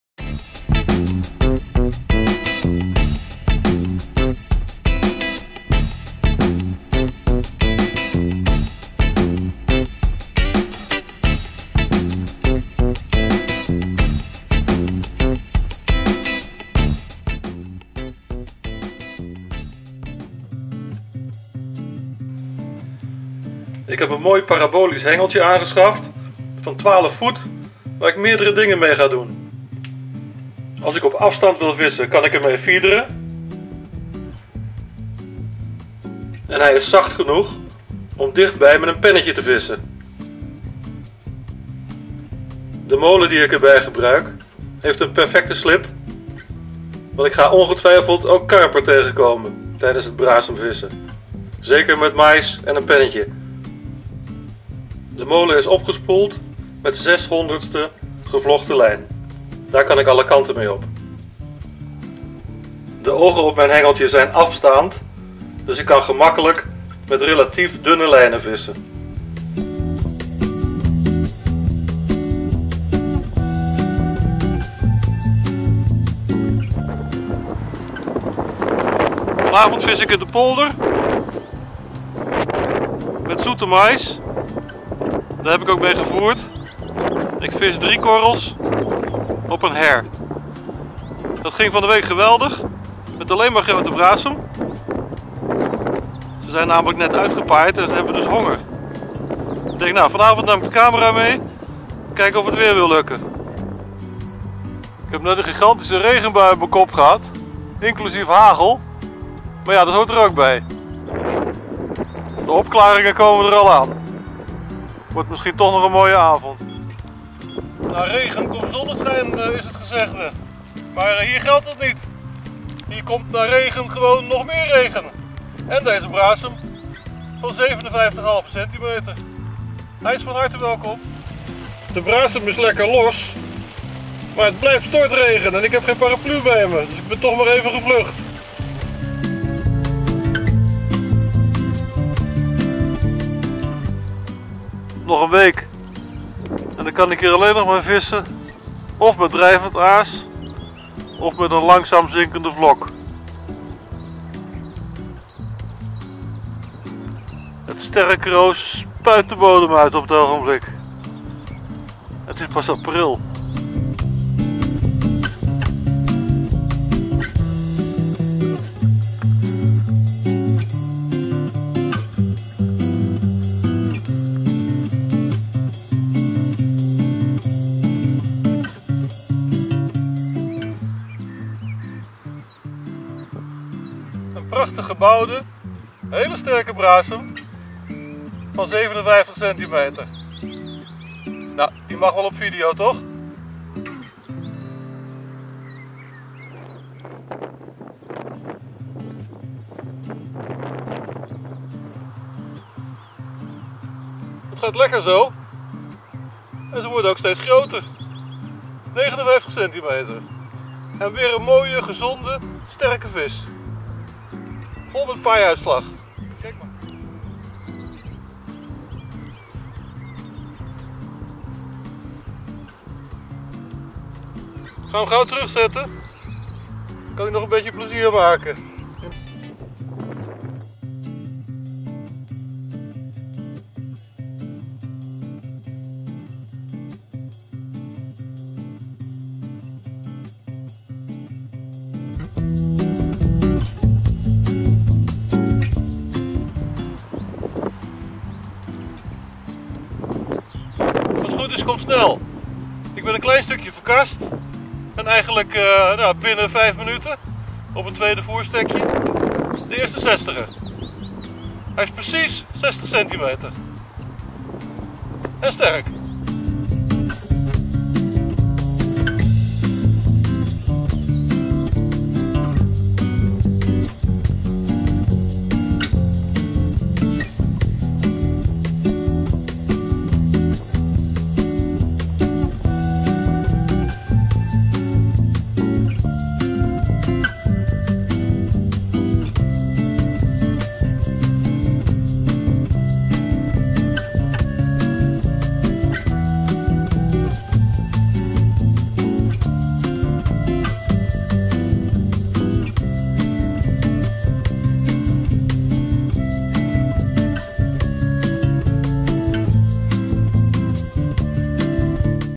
BRASEM Vissen met de Pen
Compilatie van penvissen op brasem. Het in de intro getoonde hengeltje heb ik overigens niet gebruikt. Wel een andere penhengel. Inmiddels is de  'carbon active' ingeruild voor een 100% feederhengel.
Eerder:
Pennetje + blikmais = 6 brasems van 57+ vol met paaiuitslag. Al jaren hier geen enkele kleine brasem meer gevangen, hetgeen een teken aan de wand is. Deze grote vissen zijn ca. 10 jaar oud en hebben gemiddeld nog zo'n 5 jaar te leven. Ik vrees de laatste generatie op dit water. De brasem sterft op veel plaatsen uit door overbevissing (beroep), aalscholvers, wanbeheer, KRW-richtlijnen, de Quaggamossel en andere voedselconcurrenten. Triest, maar er zijn/waren partijen die kraanwater in de sloten willen/wilden.